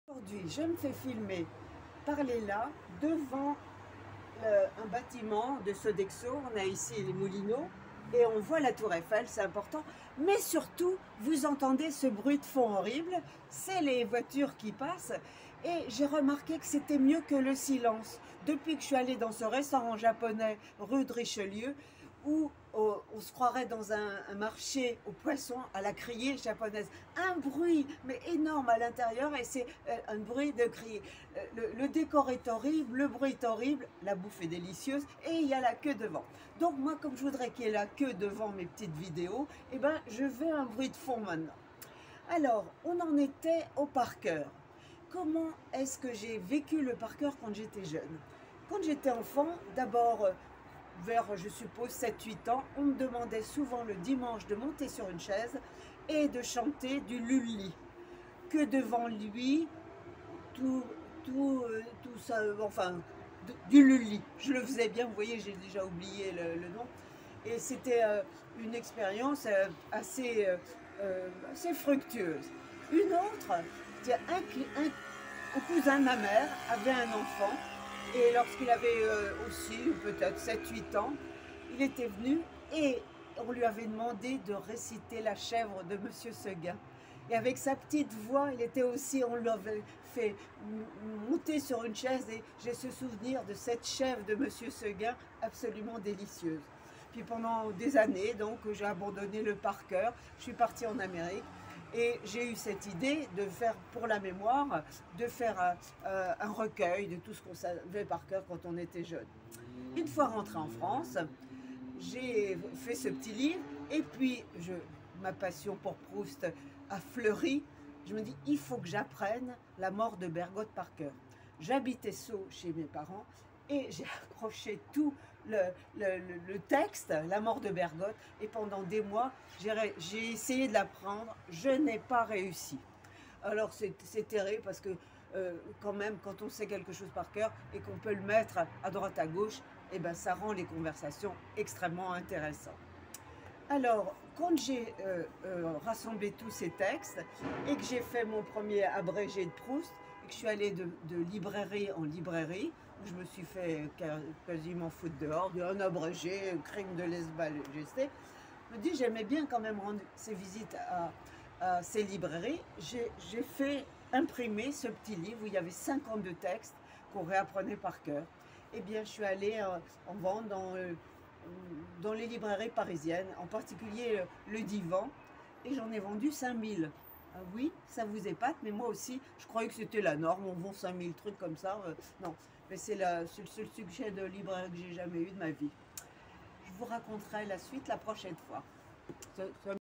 Journal d'une femme de lettres: PAR CŒUR 2
Leyla Guz me filme sur son balcon. J'apprécie le bruit de fond-façon-restaurant-japonais, je raconte des expériences de par coeur de mon enfance, jusqu'à mon petit livre à spirales